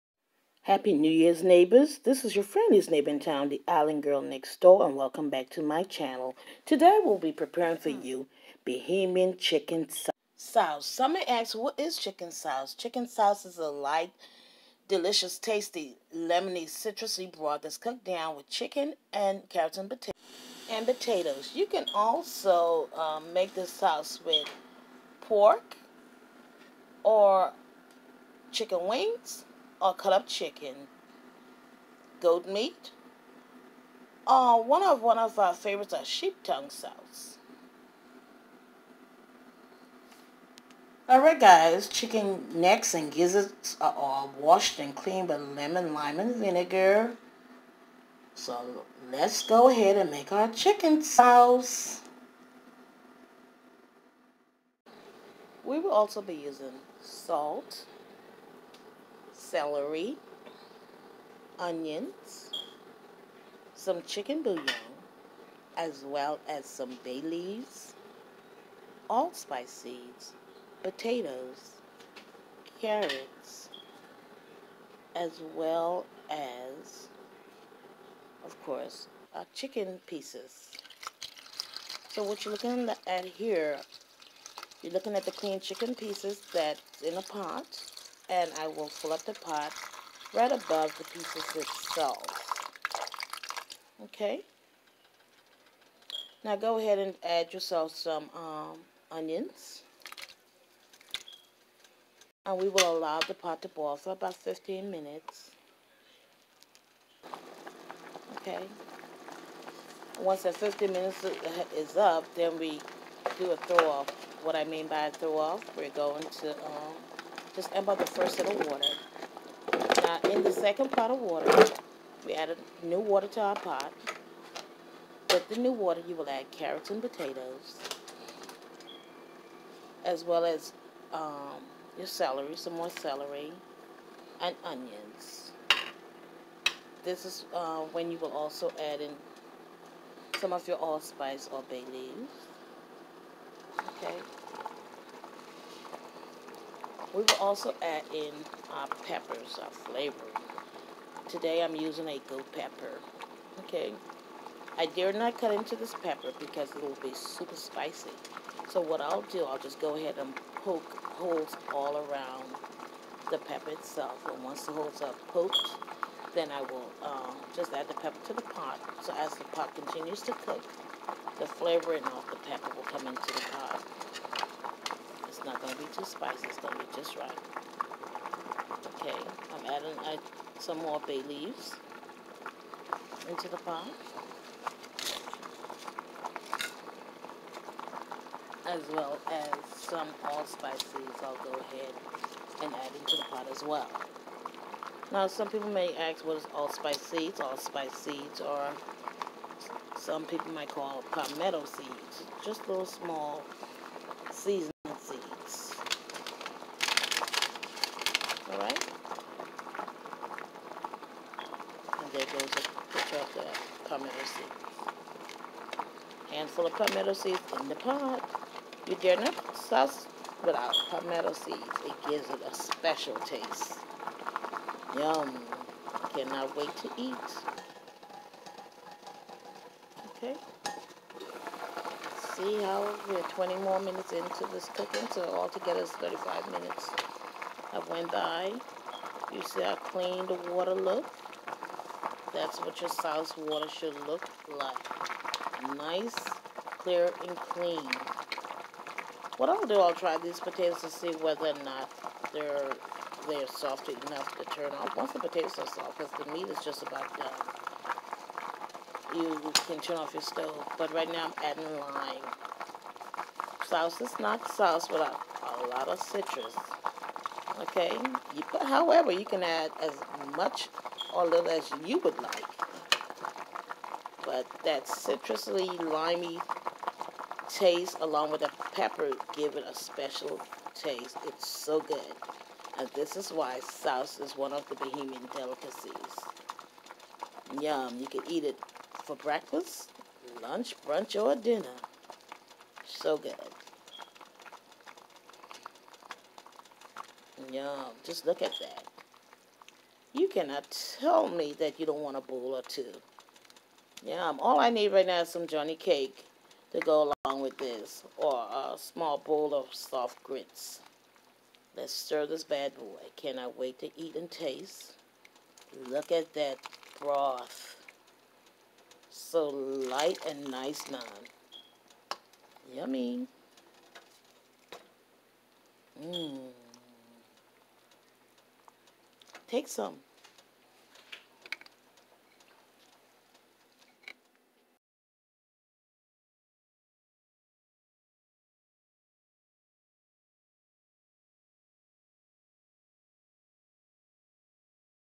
The Best Bahamian Chicken Souse
Chicken Souse Ingredients:

Chicken gizzards and necks
Potatoes
Yellow Onion
3 Celery Stalks
Carrots
Whole Cloves
Bay Leaves
Salt
Garlic and onion powder
Peppers (goat, habanero, bird)
Lime and or Lemons
Serve with Johnny Cake or Over grits and or rice or just eat as is.